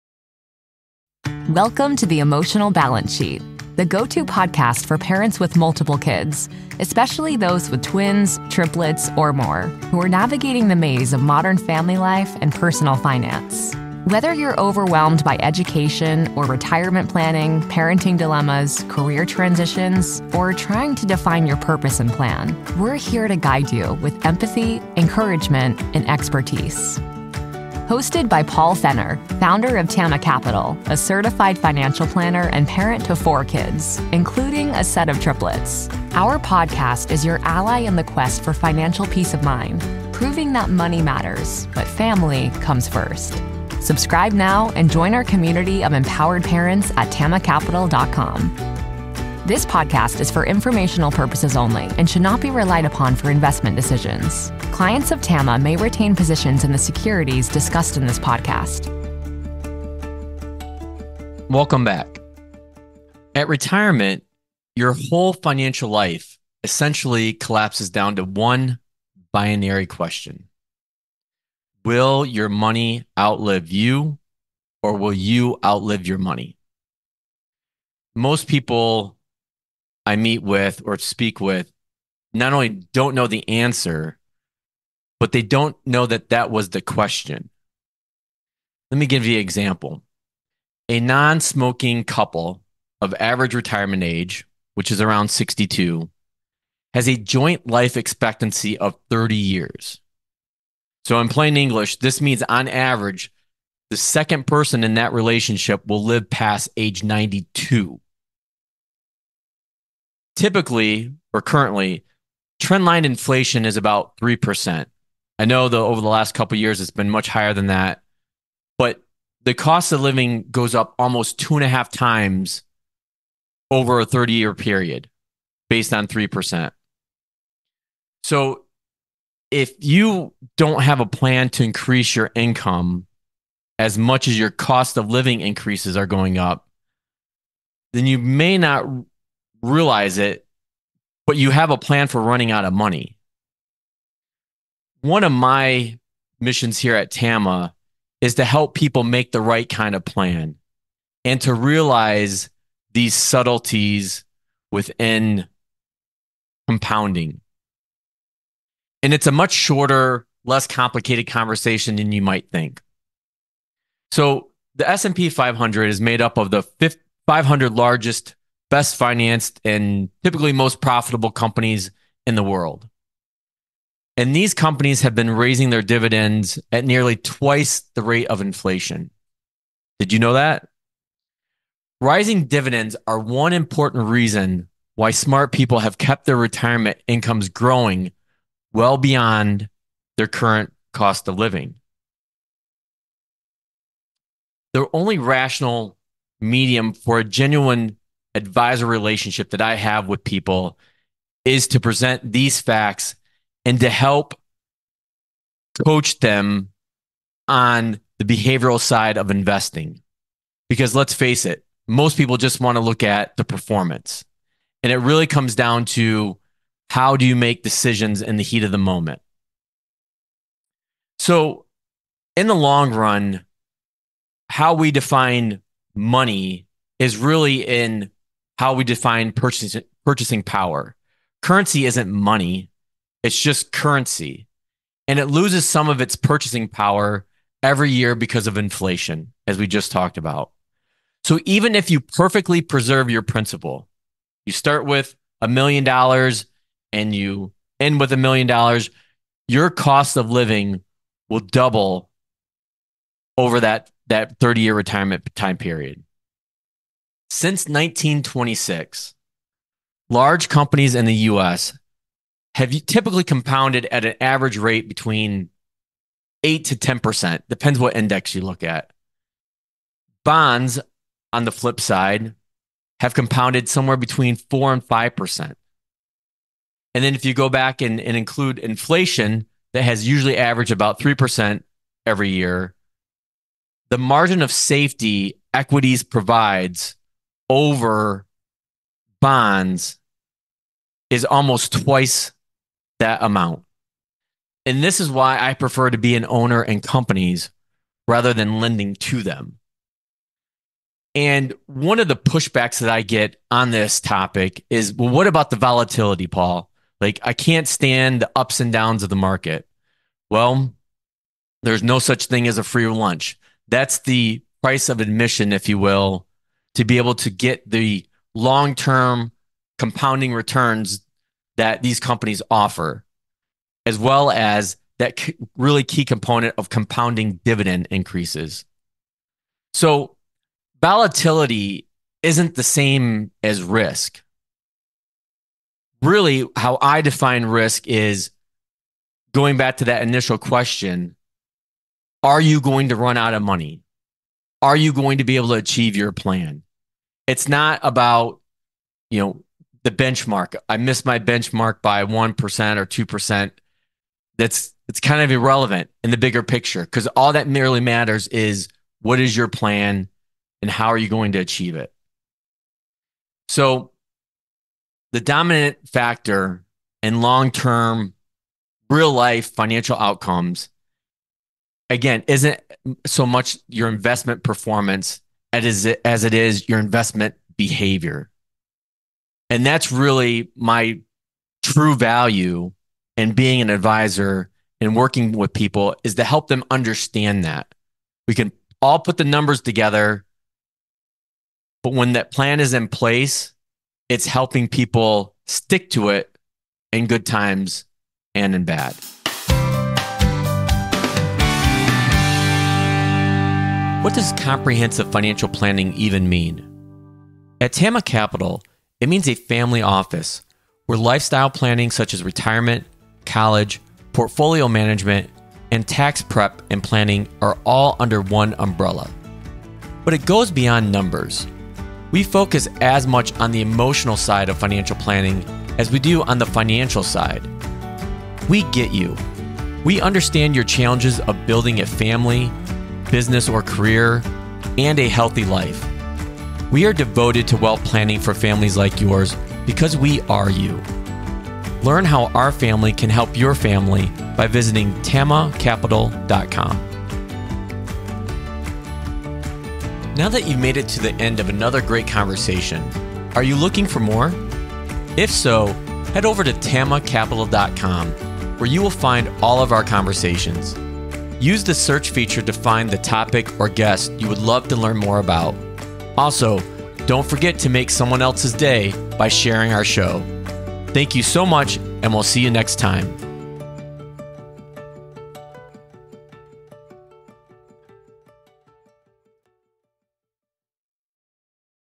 Rising Dividends
ubscribe to the show on  Apple Podcast |Google Podcast |  Amazon Music | Spotify

 At retirement, your whole financial life essentially collapses down to one binary question: will your money outlive you, or will you outlive your money?

 Volatility in the market is often seen as a risk, but it's important to differentiate between the two. The real risk lies in not having a plan that can sustain your financial needs throughout retirement. It's not about meeting a benchmark; it's about developing a plan and staying committed to it.

 The dominant factor in long-term financial outcomes isn't just your investment performance – it's your investment behavior.

 Follow Paul on Facebook and LinkedIn.